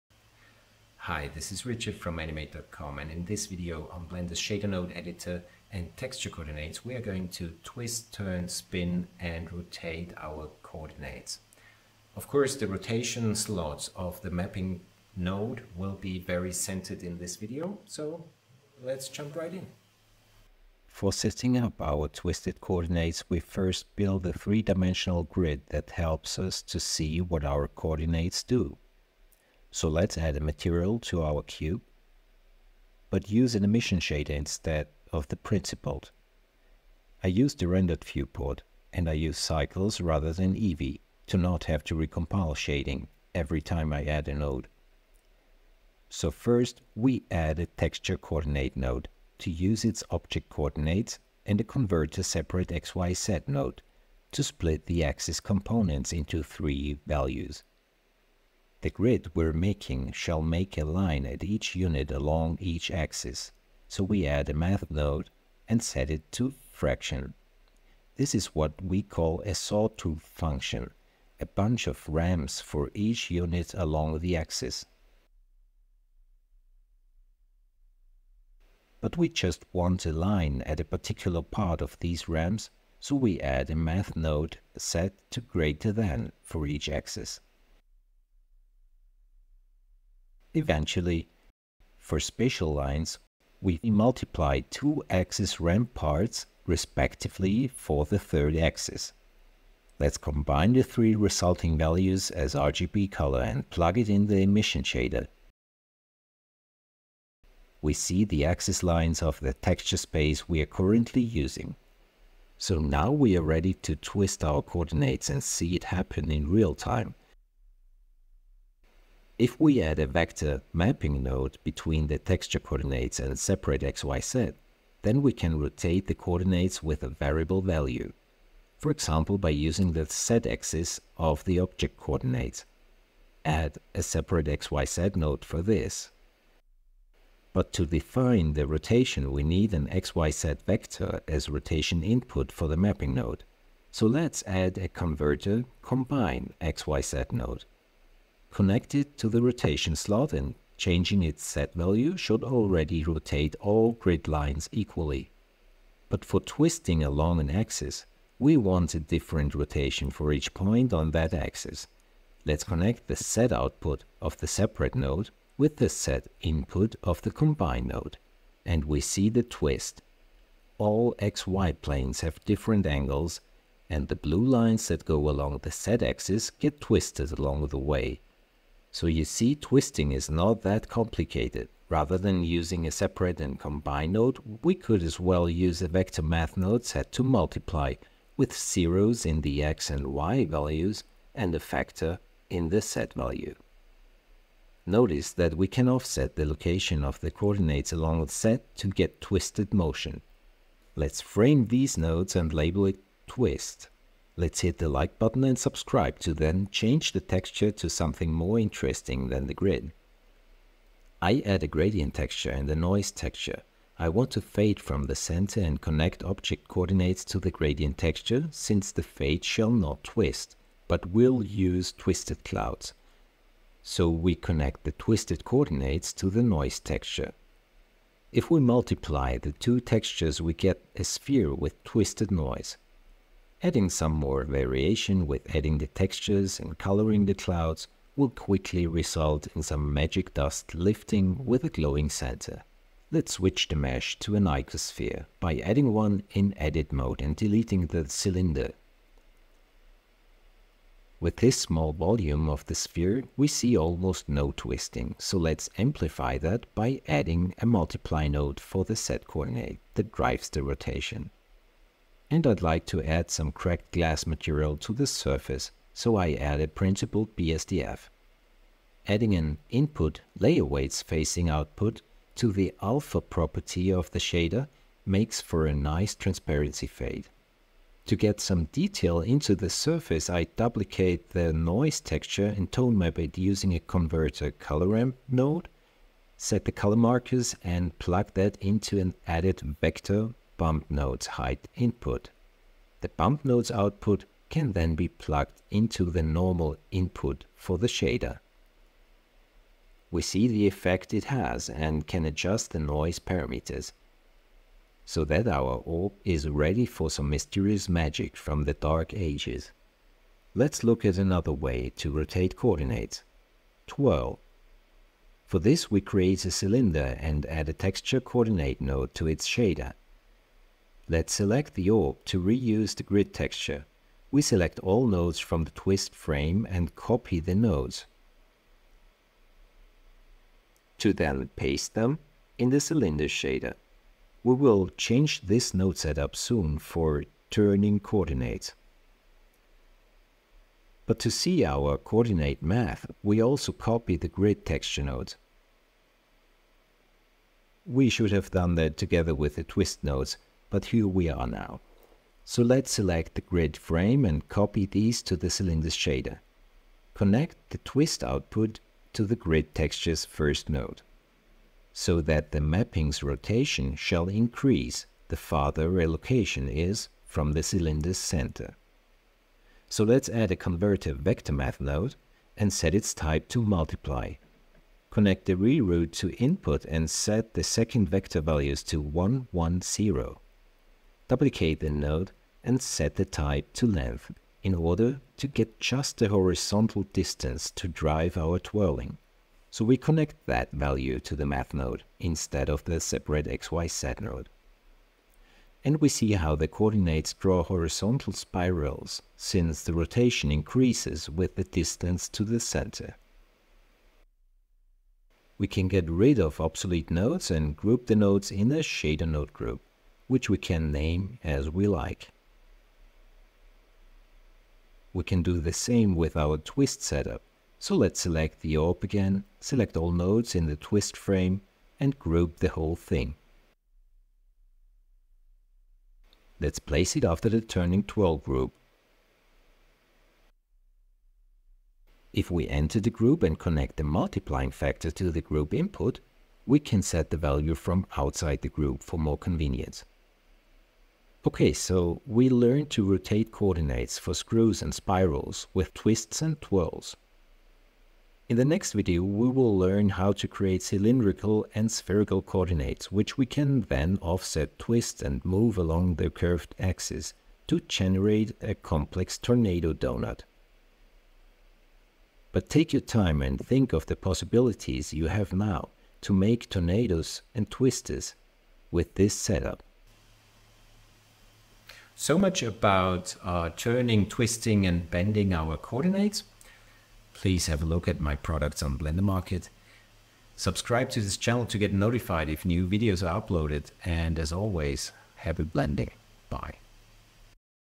ULTIMATE GUIDE to texture coordinates in blender #9 - twists and twirls
In this instructural video series we explore the methods and principles behind Blenders shadernode editor.
Lesson 9 is looking at rotation of coordinates for twists and twirls.